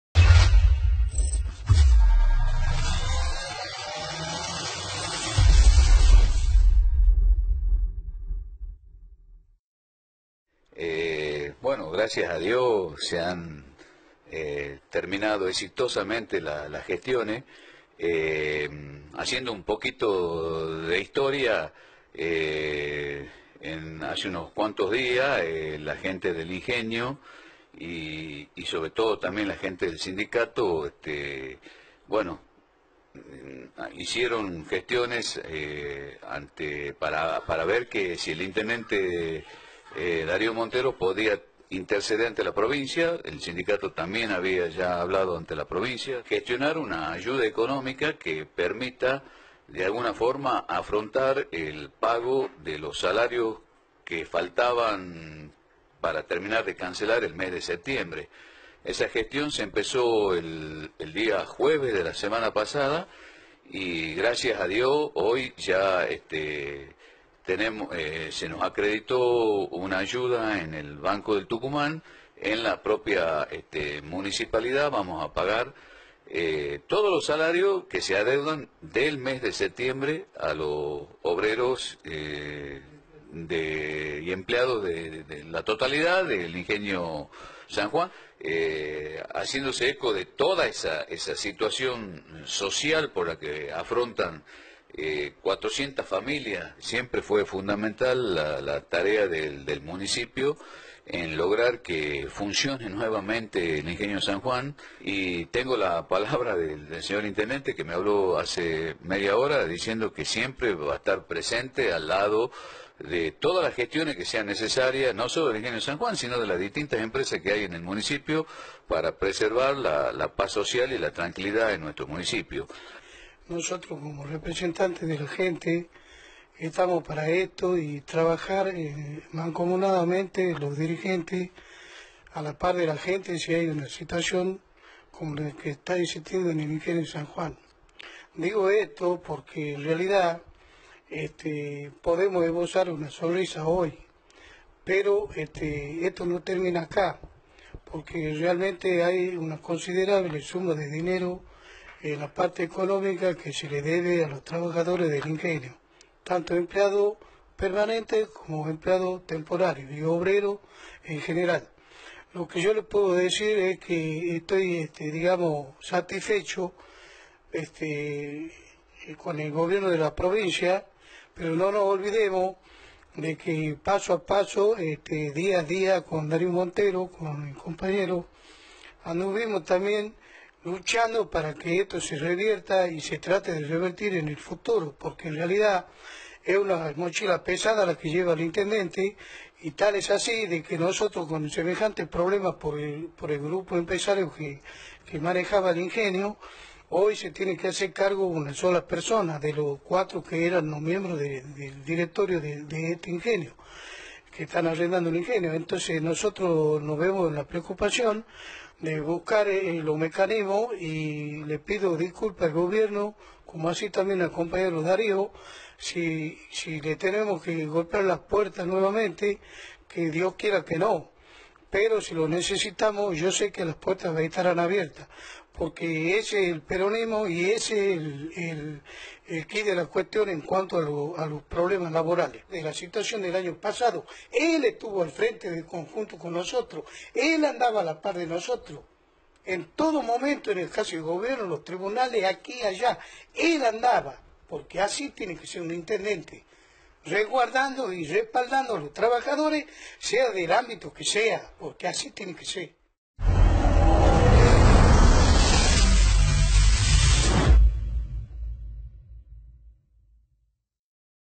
Obreros del Ingenio San Juan percibieron el pago
INGENIO SAN JUAN |En la tarde de ayer los obreros del Ingenio San Juan percibieron el pago de sus salarios, correspondiente al mes de Septiembre.

El Intendente Darío Monteros junto al Sec. Gral. del Sindicato de los Trabajaros del Ingenio San Juan Sr. Juan Abregu, ante la situación que atravesaban los trabajadores por el incumplimiento del pago de los salarios, gestionaron ante el Gobierno de la Provincia de Tucumán, la solicitud de una ayuda económica para poder saldar, en un principio, el sueldo del mes de septiembre a los obreros del Ingenio San Juan.
Más de cuatrocientas familias se vieron afectadas con el retraso de la liquidación. El Mandatario Municipal y las Autoridades del Sindicato del Ingenio, trabajaron arduamente para poder acercar una solución a los trabajadores. El compromiso del Municipio de La Banda del Río Salí es acompañar y apoyar a los trabajadores que son la fuente de ingreso de una familia, como así también su rol en la sociedad contribuye con el crecimiento de Nuestra Ciudad.